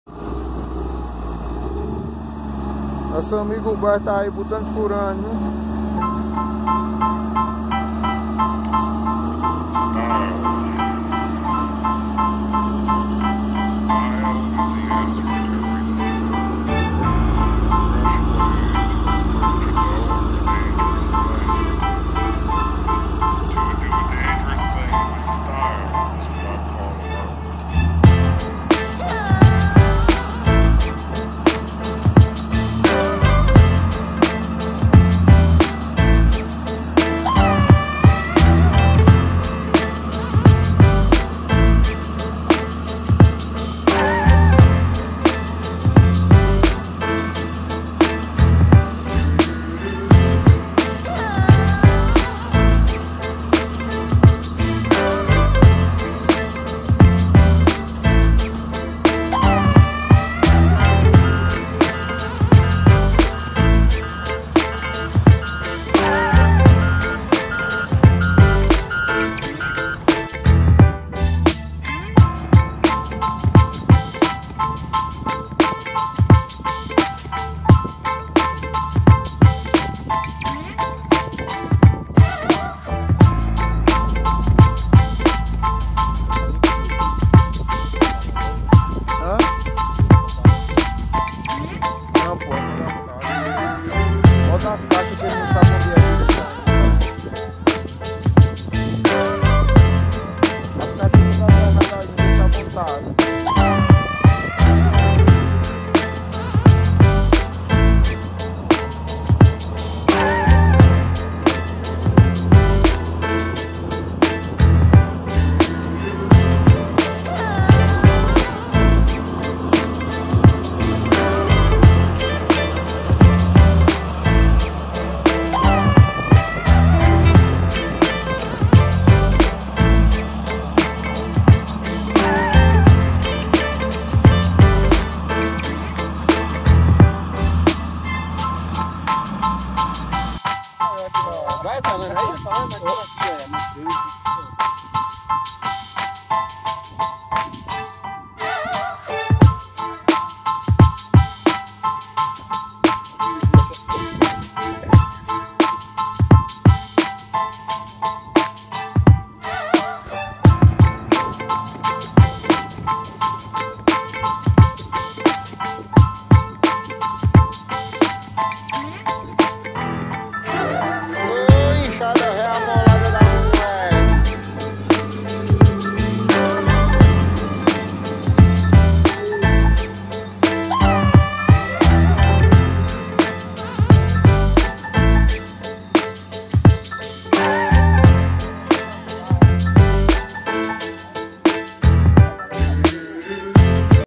Motoniveladora Case 865B, nivelando rua em loteamento.
Músico: Philip E Morris
Mestre Humberto,de campina grande-PB, botando pressão na motoniveladora Case 865B. Nivelamento de rua em loteamento.